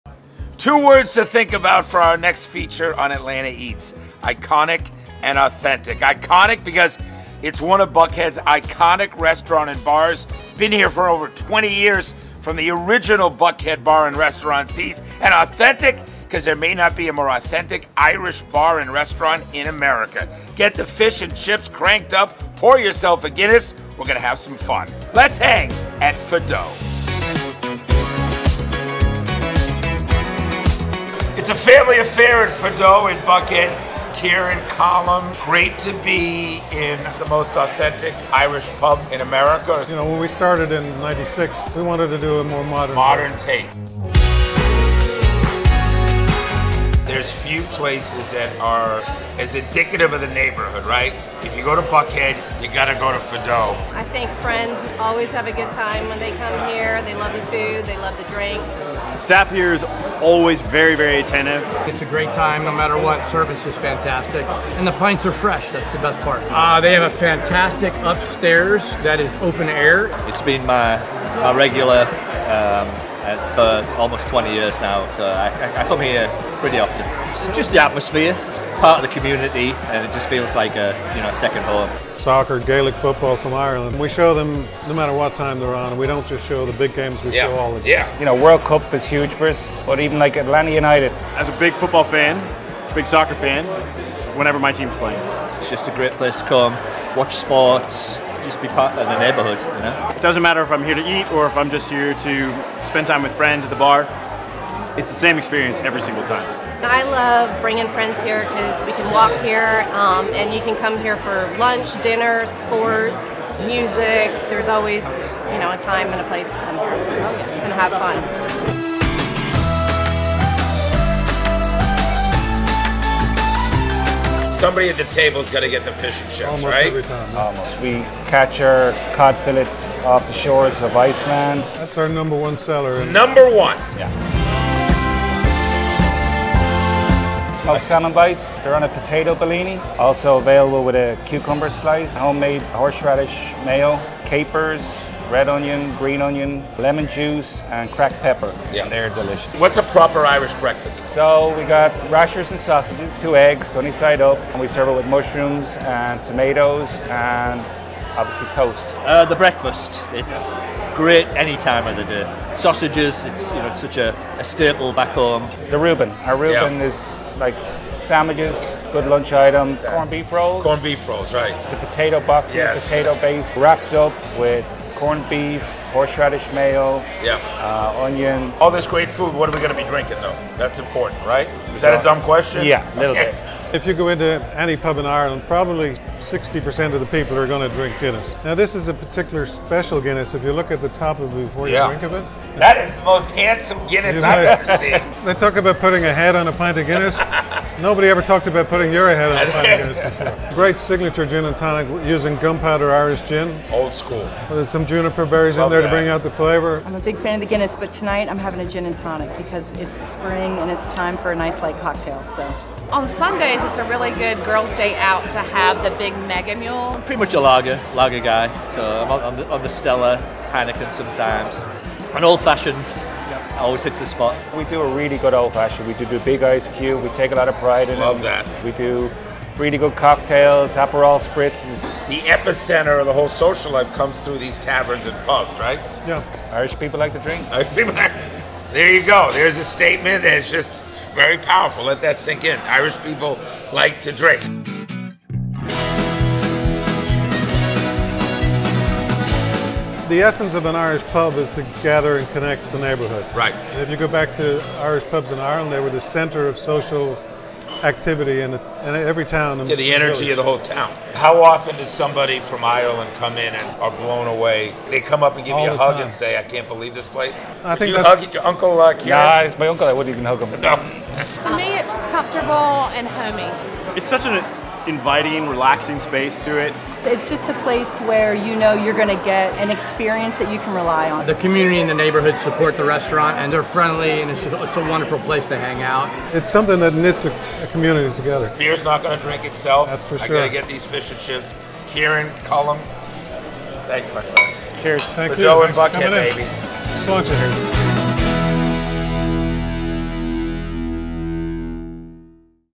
Restaurant Review - Fadó Irish Pub | Atlanta Eats
When it comes to Atlanta’s best Irish pubs, Fadó is obviously at the top of the list. But they’ve also got delicious food like Irish Breakfast, Fish and Chips, and their Smoked Salmon Bites, along with a perfectly-poured pint of Guinness. With every soccer game you could hope for on TV, huge event spaces, and true Irish roots from its founders, there are lots of reasons to learn more about Fado Irish Pub.